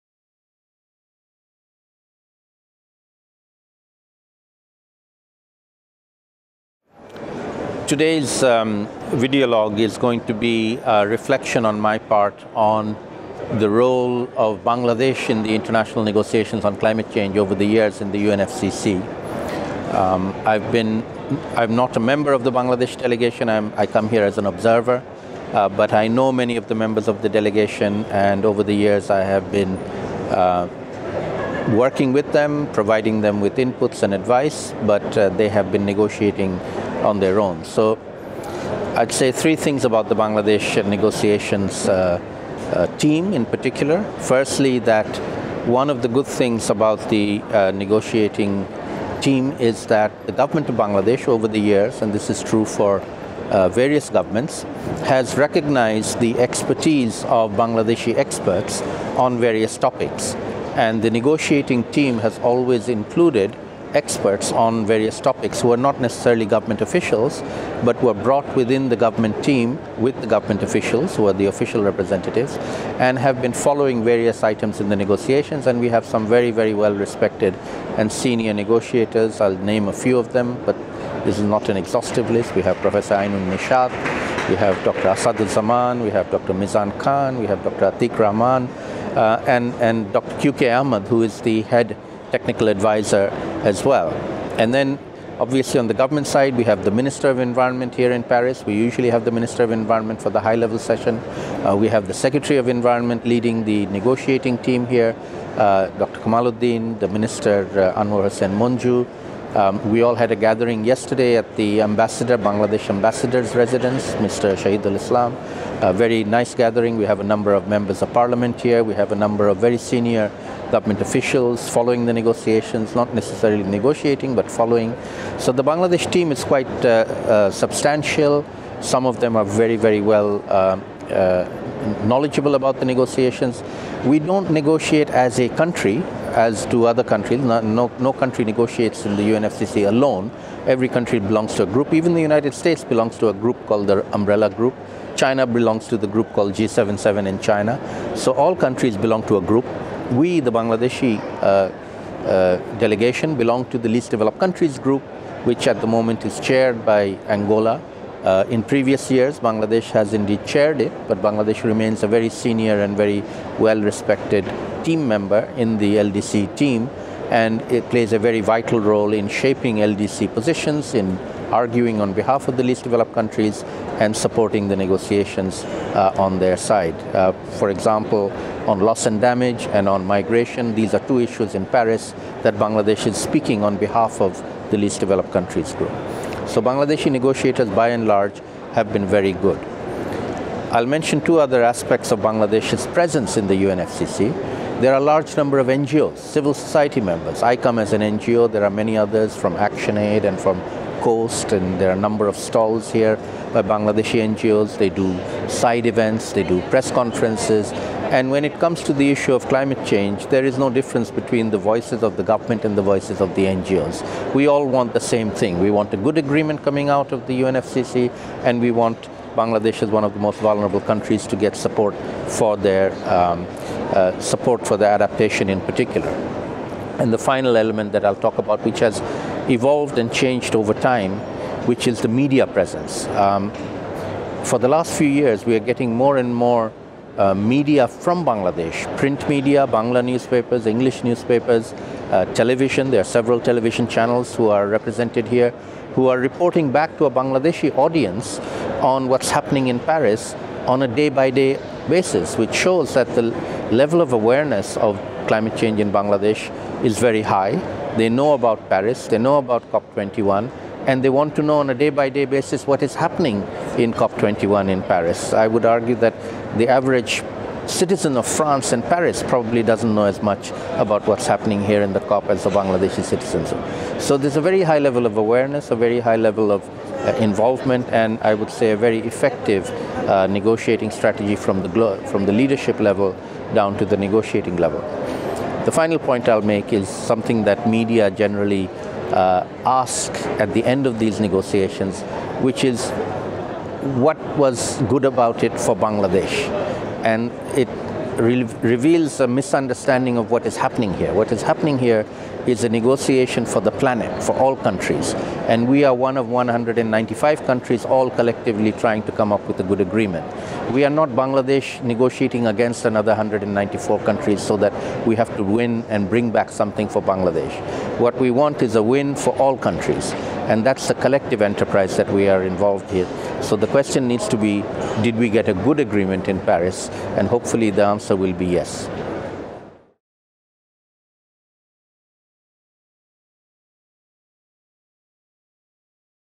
COP21 daily update with Saleemul Huq, 10 December
On the 11th day of the 21st Conference of the Parties (COP21), Dr Saleemul Huq reports on the work of the Bangladeshi delegation, as part of the Least Developed Countries Group over the years and at this COP.

A senior fellow at the International Institute for Environment and Development (IIED) and director of the International Centre for Climate Change and Development (ICCCAD).

Huq will be providing daily updates from COP21 throughout the conference.

and on Twitter via @IIED and COP21.